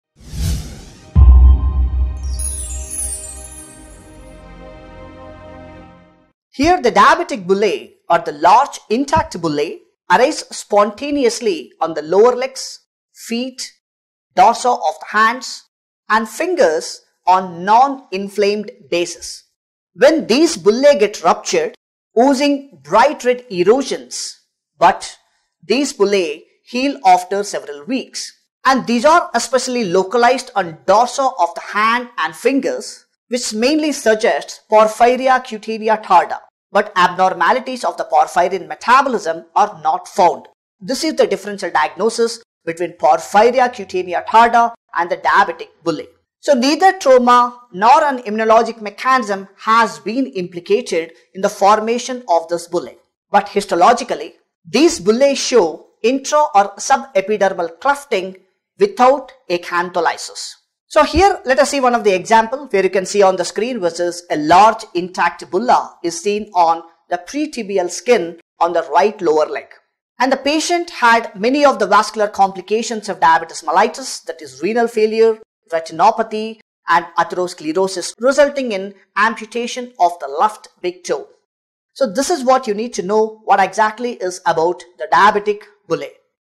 Diabetic bullae (Bullosis Diabeticorum) - Clinical essentials : Dr G Bhanu Prakash
Diabetic bullae (Bullosis Diabeticorum) - Clinical essentials

Bullous disease of diabetes (bullosis diabeticorum) is a distinct, spontaneous, noninflammatory, blistering condition of acral skin that is unique to patients with diabetes mellitus. Bullous disease of diabetes tends to arise in long-standing diabetes or in conjunction with multiple complications. The etiology of the disease is yet unknown. In the United States, bullous disease of diabetes has been reported to occur in approximately 0.5% of diabetic patients. Male patients have twice the risk as female patients.

Kramer first reported bullous-like lesions in diabetic patients in 1930 ; Rocca and Pereyra first characterized this as a phlyctenar (appearing like a burn-induced blister) in 1963.  Cantwell and Martz are credited with naming the condition bullosis diabeticorum in 1967.  It is also termed bullous disease of diabetes and diabetic bullae.

While lesions typically heal spontaneously within 2-6 weeks, they often recur in the same or different locations. Secondary infections may also develop; these are characterized by cloudy blister fluid and require a culture.

The clinician should consider direct immunofluorescence (DIF) studies to exclude histologically similar entities (eg, noninflammatory bullous pemphigoid, epidermolysis bullosa acquisita, porphyria cutanea tarda, other bullous porphyrias), as DIF studies are only rarely positive in bullosis diabeticorum.

Pseudoporphyria blistering due to photosensitizing drugs, chronic dialysis regimens, or ultraviolet A tanning devices should also be considered.

Specific treatment is unwarranted unless secondary infections (eg, staphylococcal) occur, thereby warranting antibiotic therapy. However, aspiration of fluid from lesions using a small-bore needle might help prevent accidental rupture.